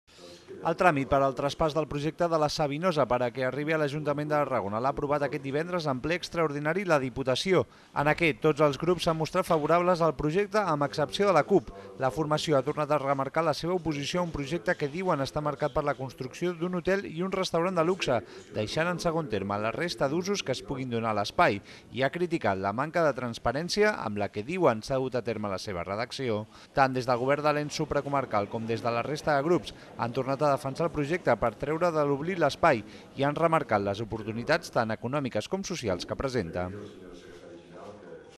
Només la CUP s'oposa al projecte de la Savionsa a la Diputació
El tràmit per al traspàs del projecte de la Savinosa per a que arribi a l'Ajuntament de Tarragona l'ha aprovat aquest divendres en ple extraordinari la Diputació. En aquest, tots els grups s'han mostrat favorables al projecte amb excepció de la CUP. La formació ha tornat a remarcar la seva oposició a un projecte que diuen està marcat per la construcció d'un hotel i un restaurant de luxe, deixant en segon terme la resta d'usos que es puguin donar a l'espai i ha criticat la manca de transparència amb la que diuen s'ha dut a terme la seva redacció